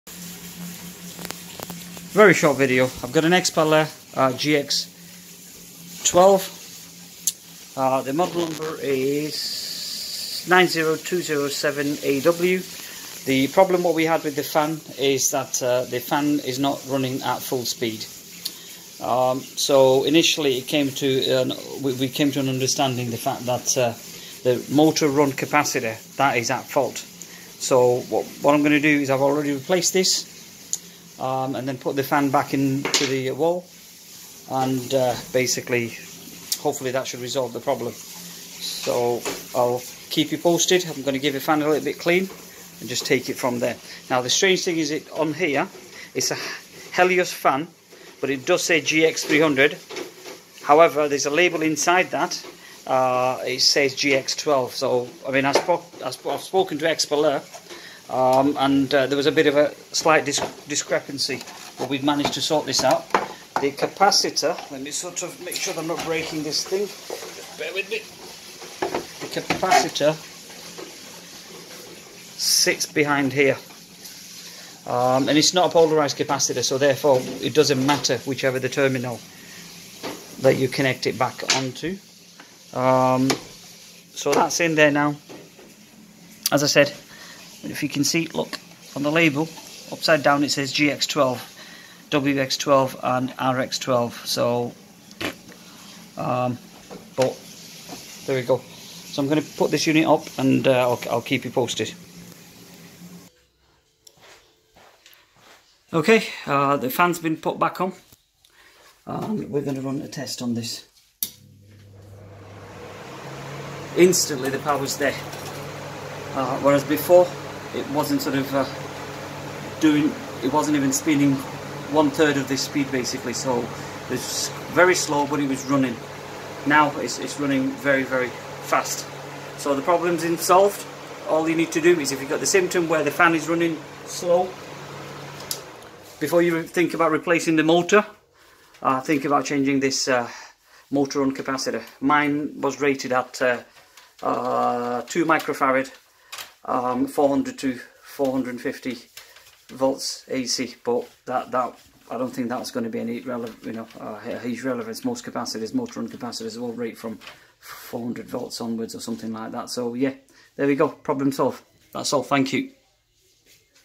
HOW TO FIX XPELAIR GX15/GX300 SLOW FAN SPEED REPLACE 2UF MOTOR RUN CAPACITOR
XPELAIR GX15/GX300 SLOW FAN SPEED FIXED REPLACE 2UF MOTOR RUN CAPACITOR